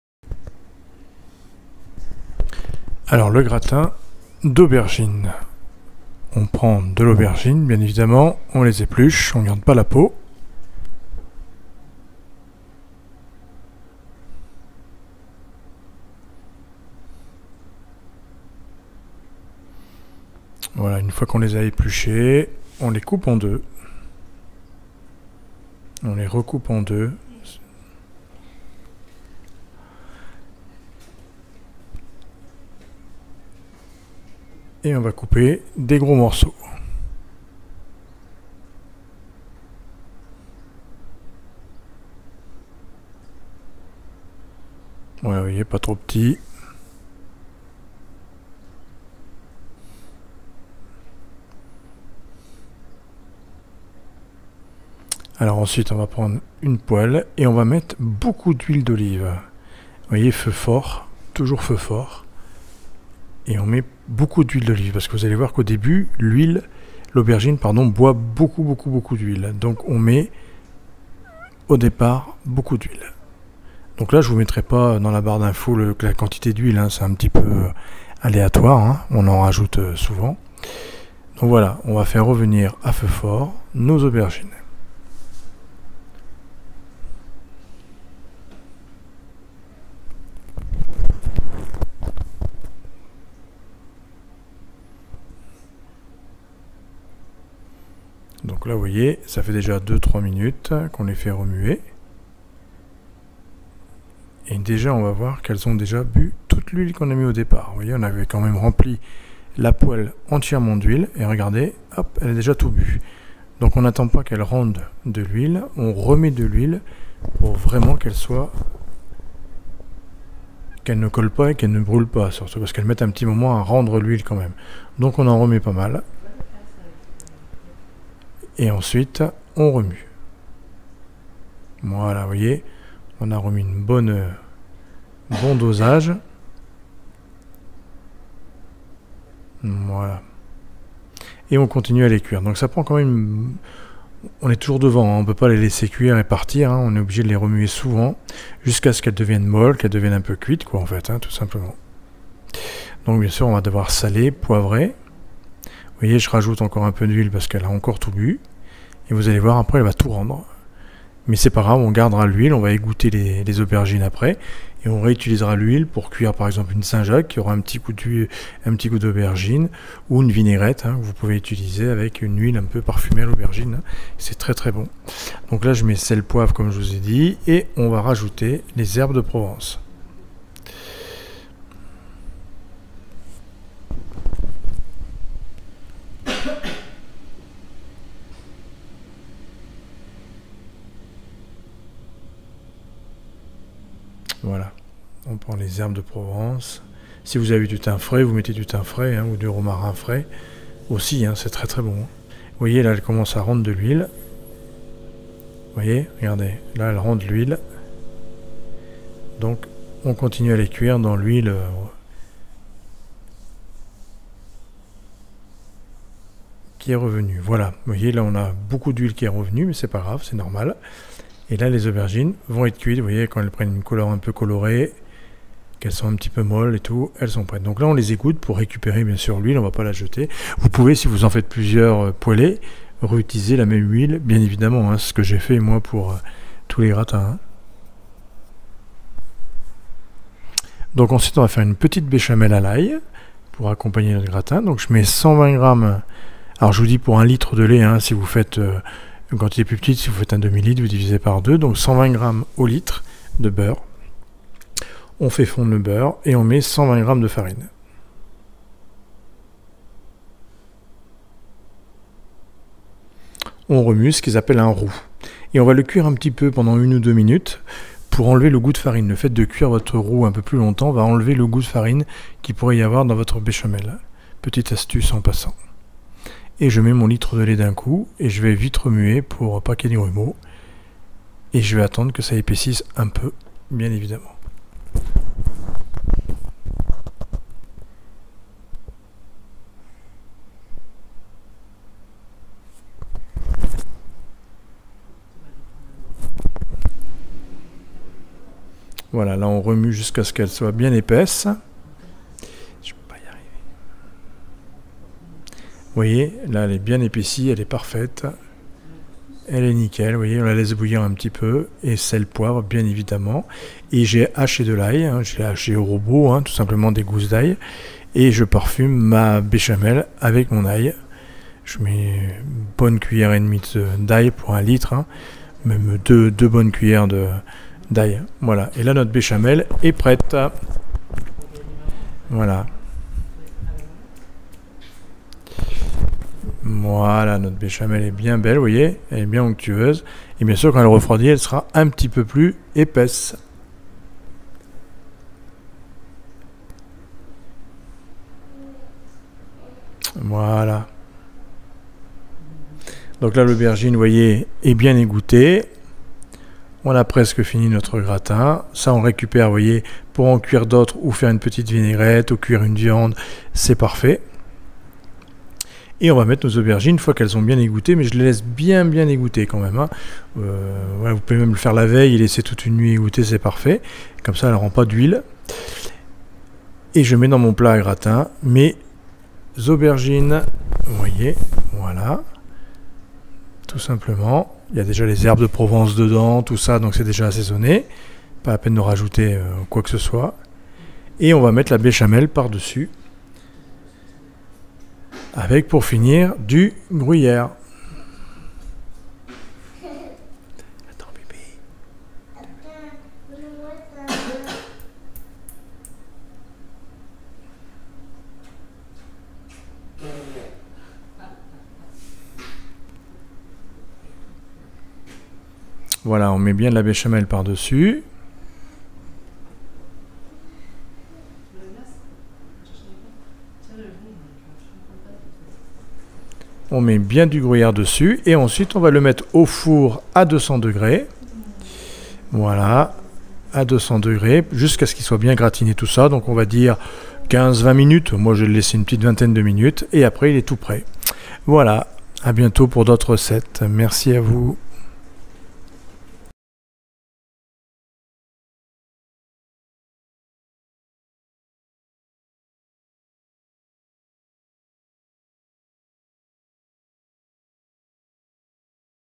GRATIN AUBERGINE
voici mon gratin d'aubergines, avec une viande c'est le top, même juste comme ca.

5 BELLES AUBERGINES.
200G HUILE D'OLIVE CA PEUT ETRE PLUS.
SEL, POIVRE, HERBES DE PROVENCE.
60 G BEURRE
60 G FARINE
1/2 LITRE DE LAIT
1 CAS AILS HACHE.
80G GRUYERE RAPE.